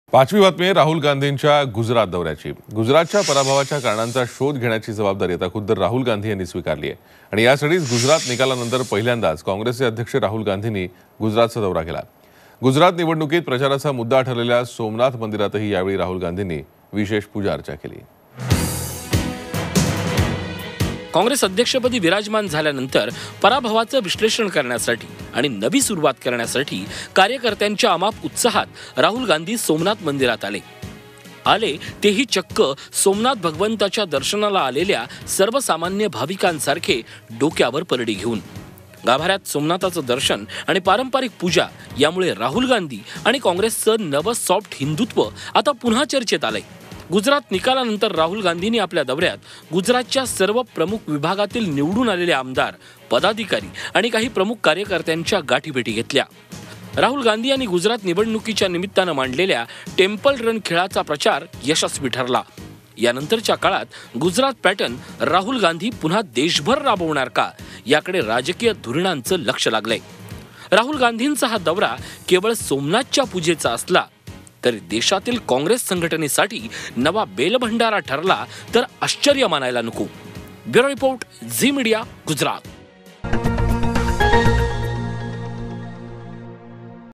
गुजरात । निकालानंतर पुन्हा राहुल गांधी गुजरात दौ-यावर
Gujrat | Congress President | Rahul Gandhis | Analyses Of Gujarat Results